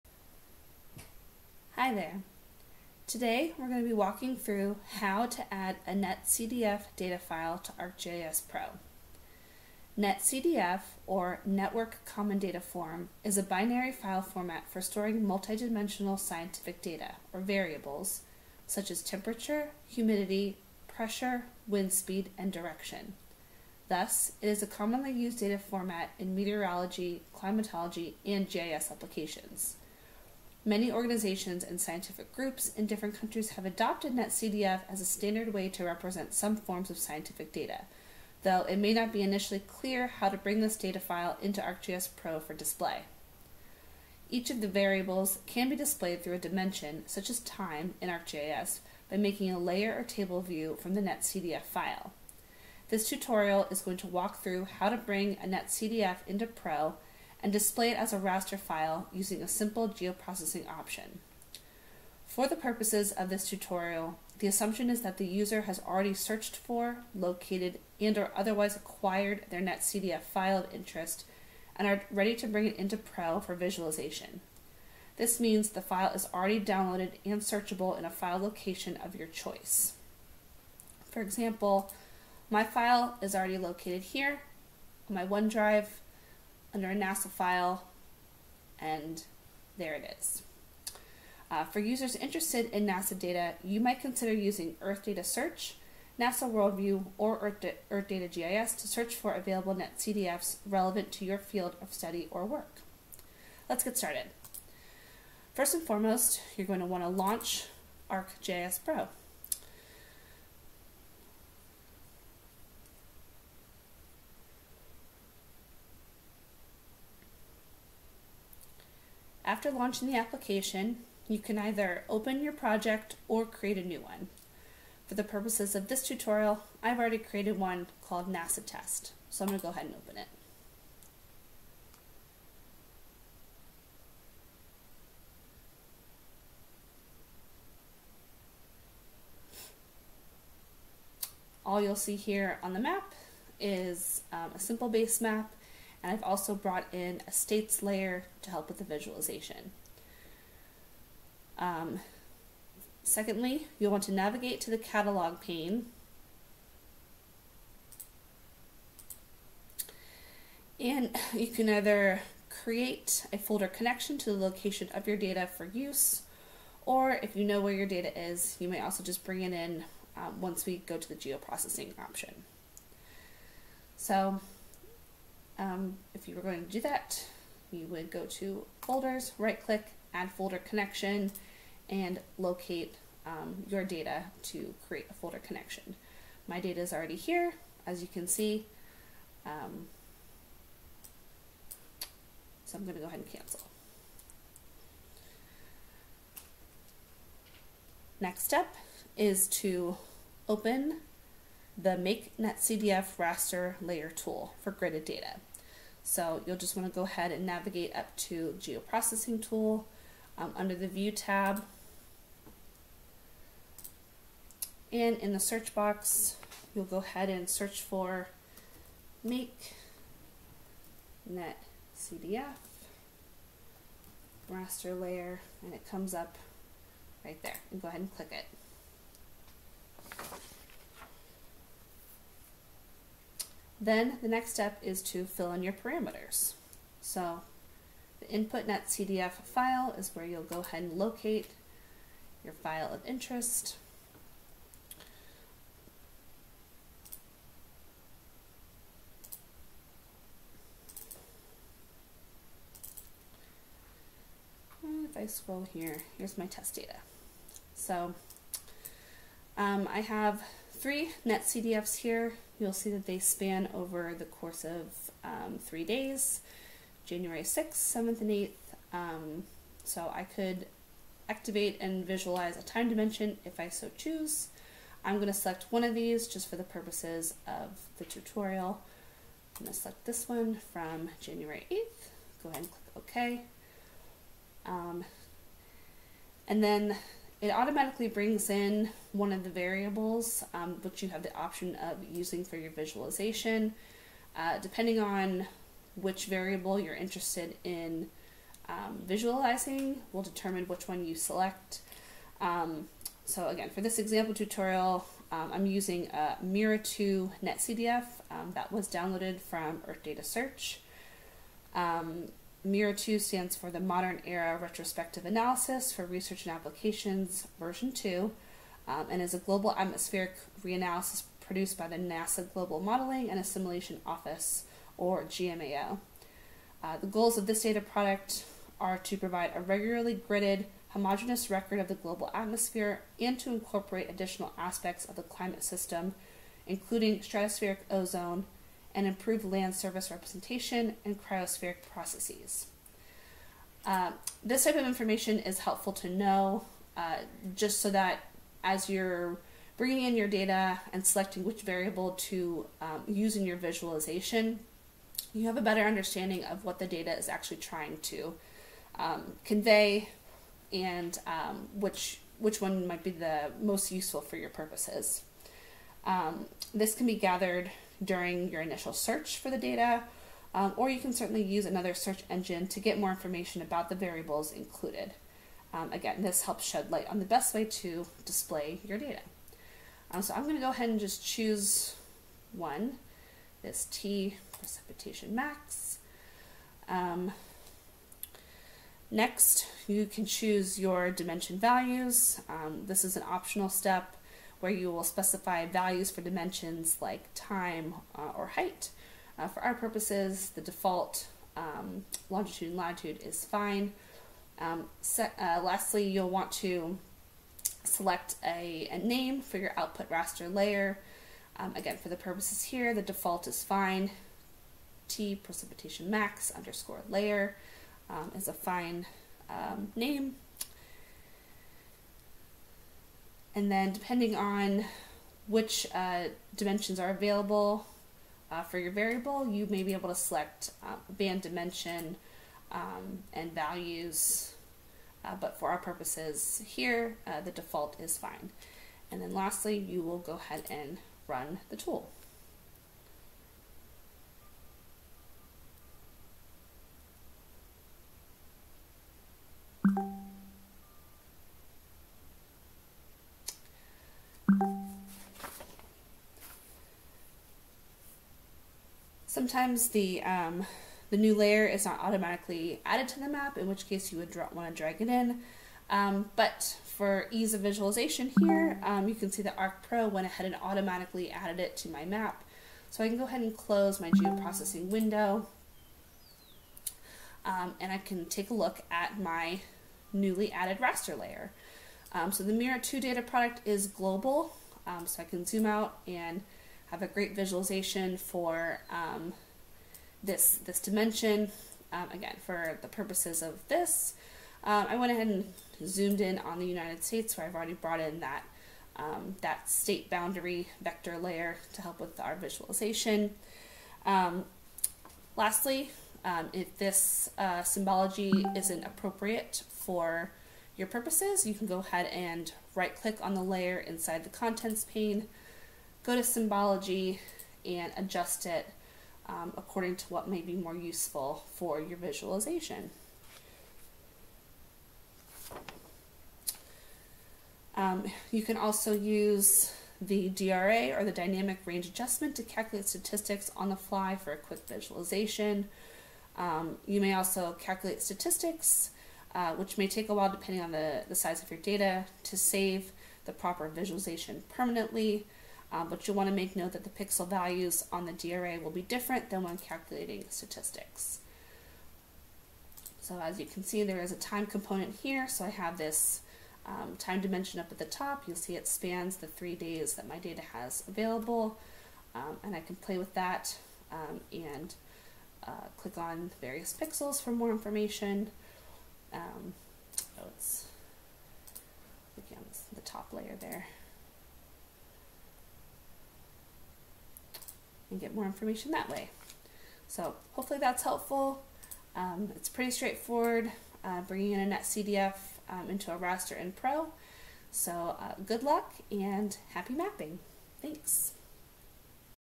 Tutorial: How to Display a NetCDF File as a Raster in ArcGIS Pro using NASA MERRA-2 Data
NetCDF is a commonly used data format within the scientific community. It stores multidimensional scientific data as variables such as temperature, humidity, pressure, wind speed, and direction. NetCDF files are commonly utilized in meteorology, climatology, and GIS applications. These files, though standard in representing some forms of scientific data, may not be initially clear how to visualize them using ArcGIS Pro. In fact, each of the available variables, depending on the specific data product, can be displayed as a raster through a dimension (such as time) in ArcGIS by making a layer or table view from the NetCDF file.

In this brief tutorial, we will use an example from the MERRA-2 satellite product (.nc4) and visualize it in ArcGIS Pro using the "Make NetCDF Raster Layer" geoprocessing tool. It is recommended to do a bit of further digging into any data product's metadata and available variables to have a good understanding of what is being shown in the map.